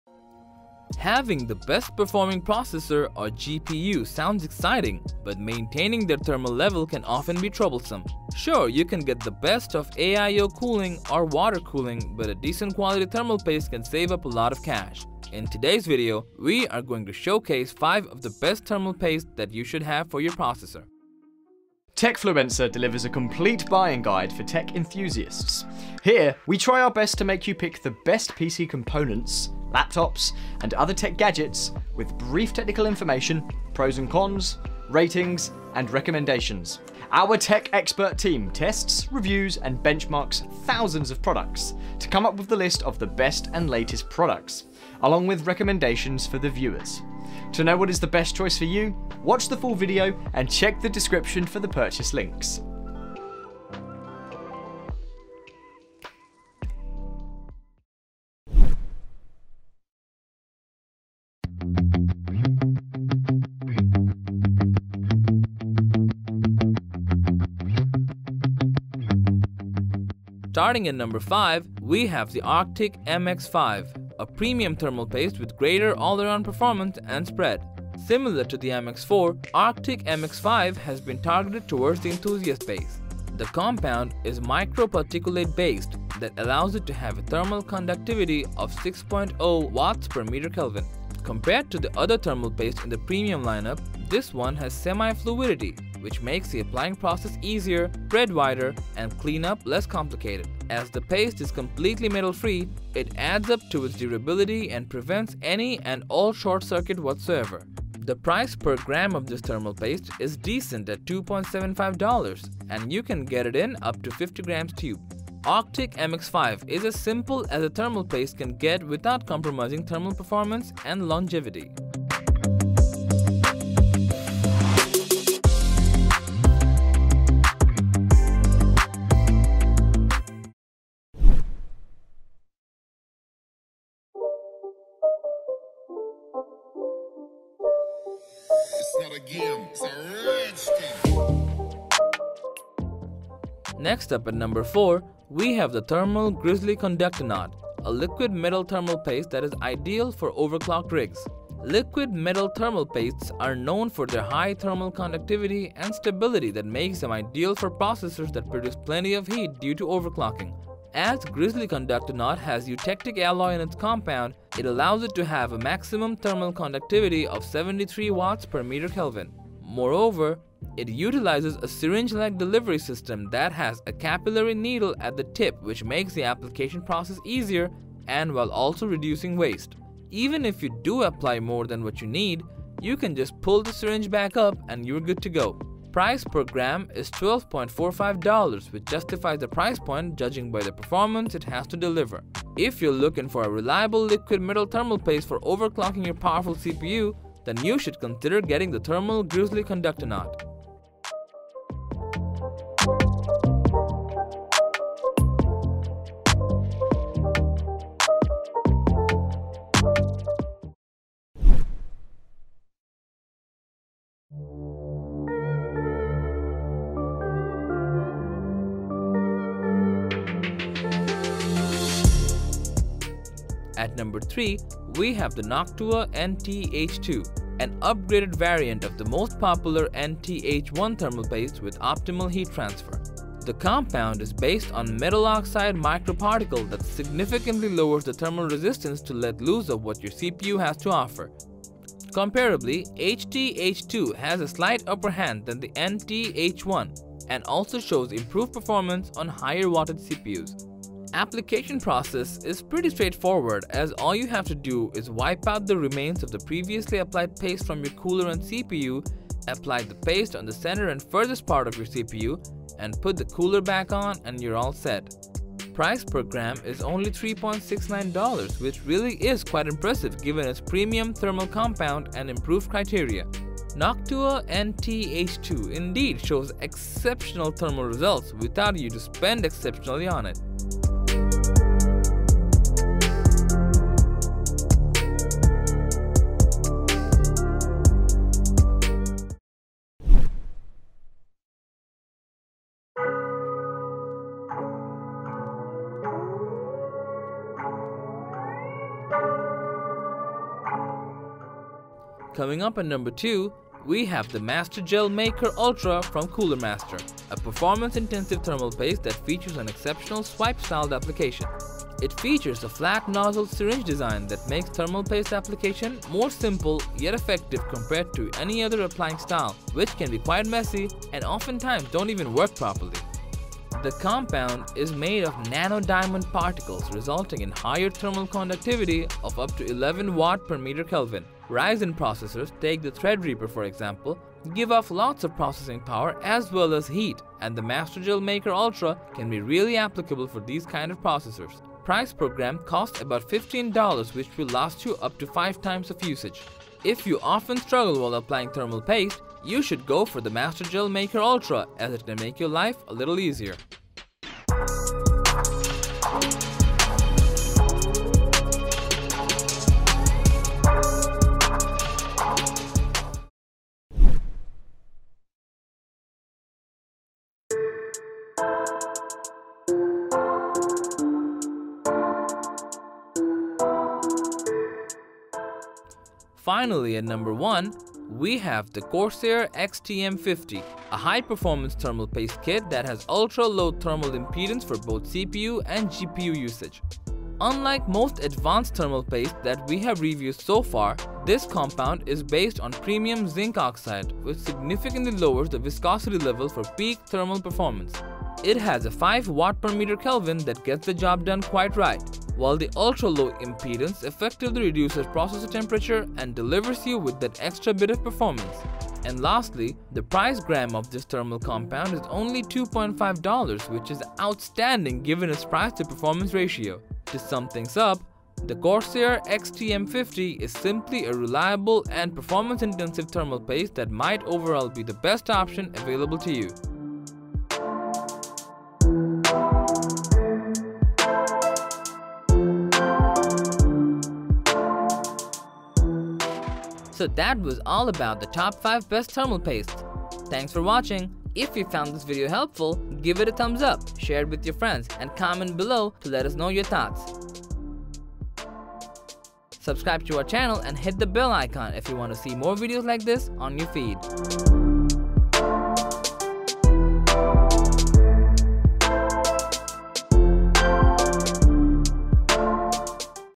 Top 5 Best Thermal Paste
Getting the best thermal paste can not only increase your processor's performance but also increase its durability. Here are the Top 5 Best Thermal Paste that you might wanna check out.

1:00 - 5. ARCTIC MX-5

2:12 - 4. Thermal Grizzly Conductonaut

3:32 - 3. Noctua NT-H2

4:50 - 2. Cooler Master MasterGel Maker Ultra

6:04 - 1. Corsair XTM50